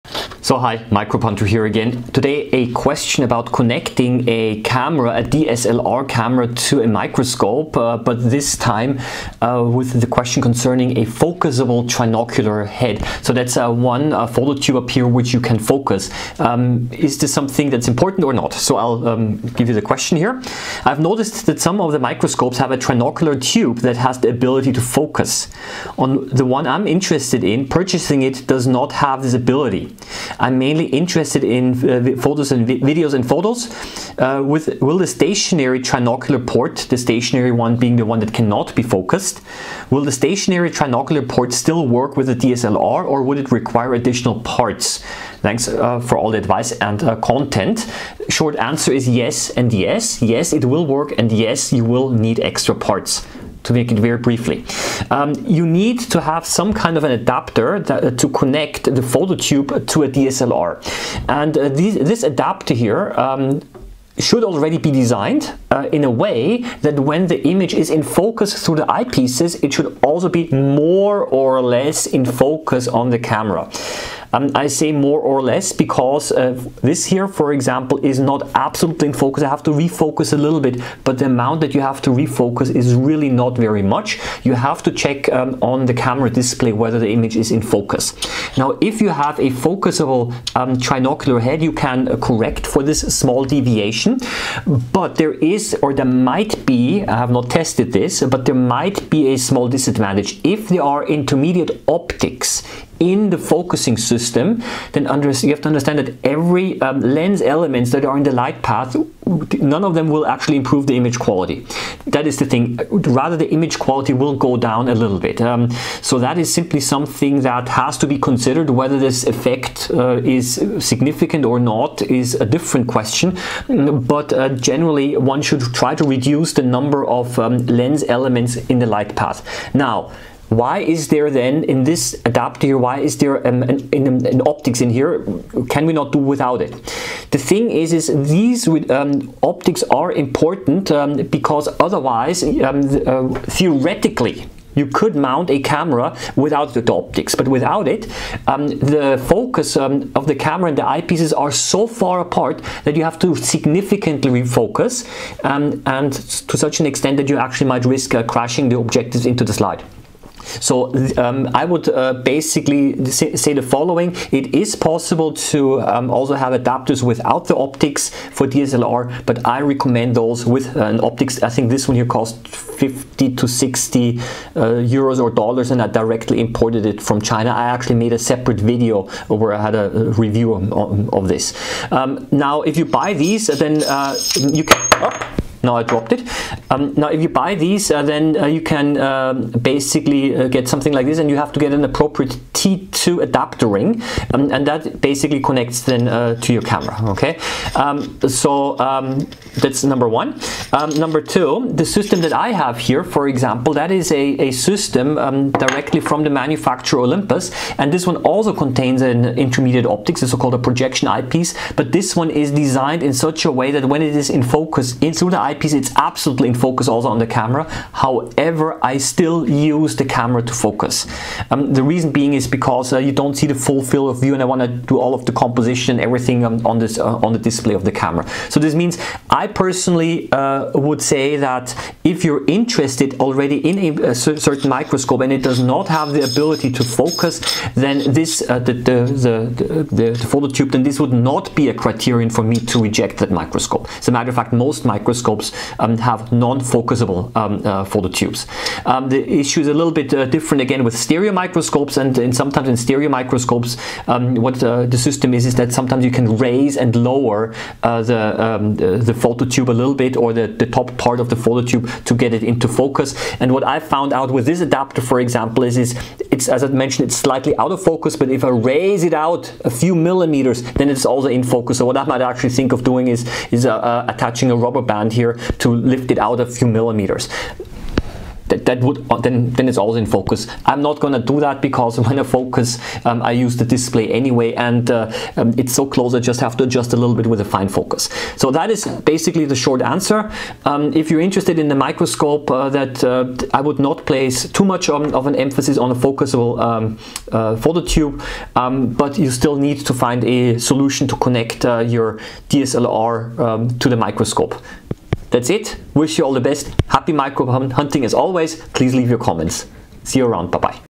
🔬 About focusable phototubes | Amateur Microscopy
Some microscoes have a phototube that can be focused. This is to ensure that the image on both the eyepiece and on the camera are in focus at the same time. Without this a small amount of refocusing might be necessary.